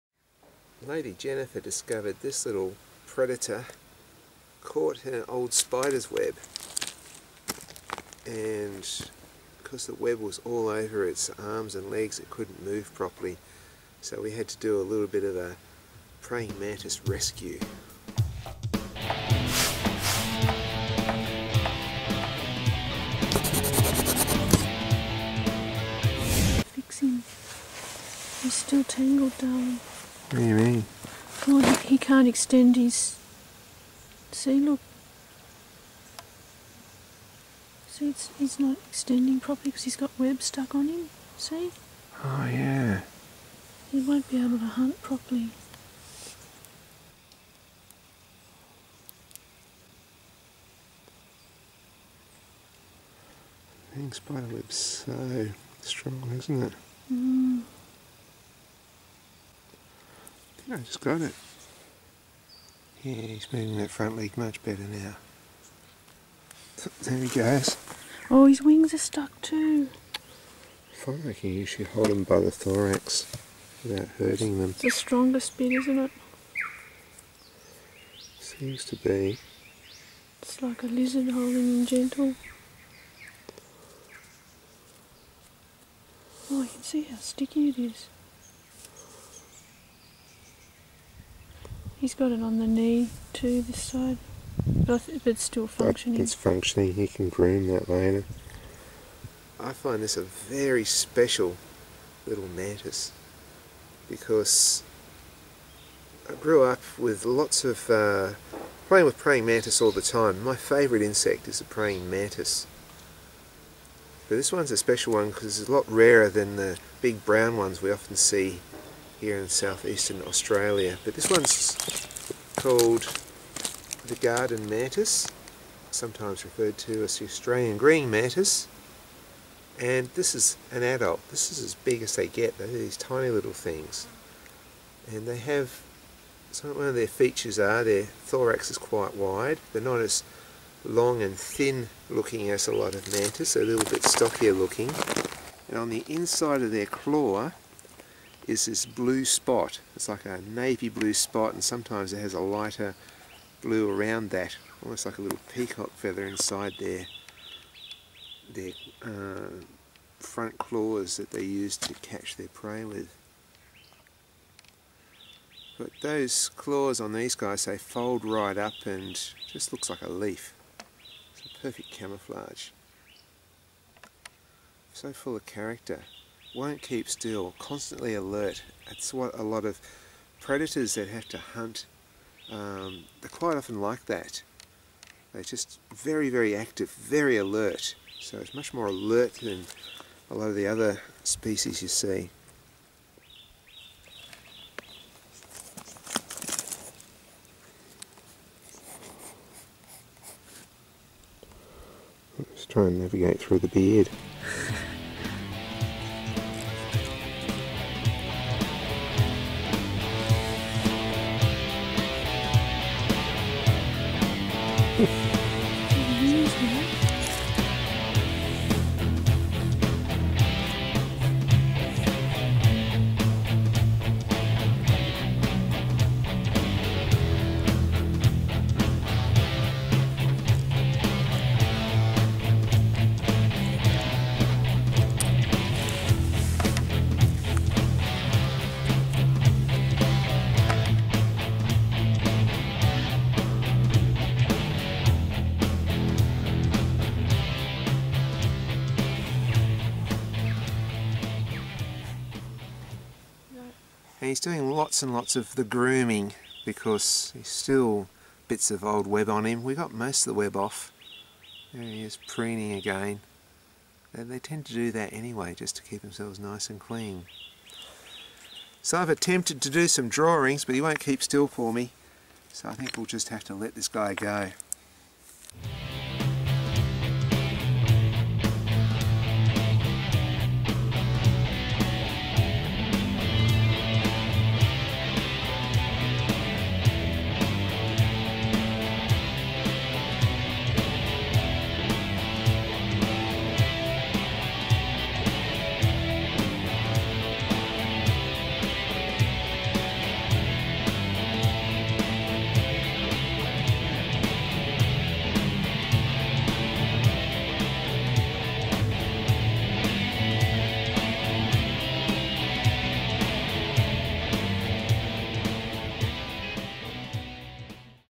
Praying Mantis, Rescued from Spider Web
Jennifer found this small  mantis caught in an old spider web.We rescued it and released it after attempting a quick drawing.

Garden Mantis (Orthodera ministralis)